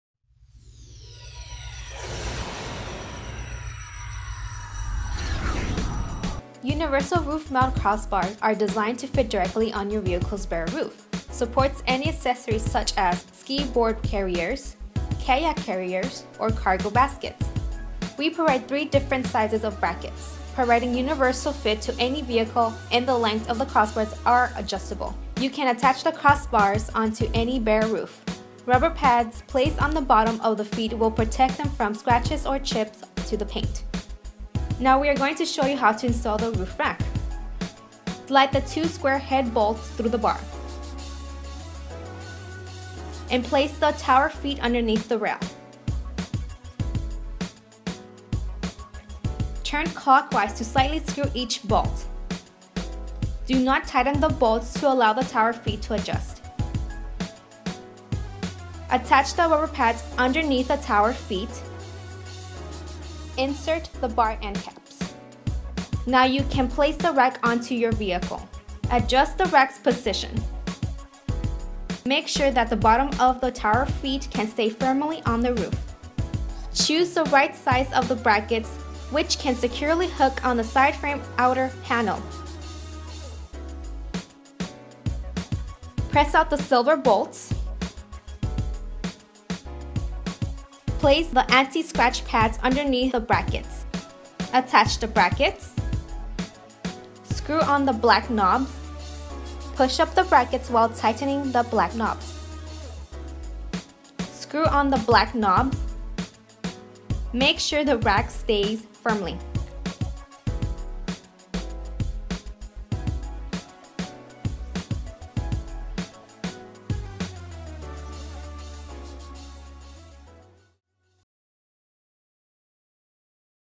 Universal Rooftop Roof Rack Crossbar Installation Guide by LT Sport CB-BU-N
This is a short video of installing rail roof top rack crossbar instruction. The luggage roof rack is inexpensive (Through Amazon,ebay, Walmart or ltsport.com) and easy to install.

Install time: 15-20 minutes.

Installation hardware and mounting accessories Included. Ultra-light weight 3mm aluminum alloy mold with a capacity of 150 lbs. Simple and easy to install. Installs and removes within minutes. No modification, cutting, nor drilling required (Adjustable Clamps for Desirable Length). Long lasting and durable finish. Suitable for all weathers. Professional installation is recommended.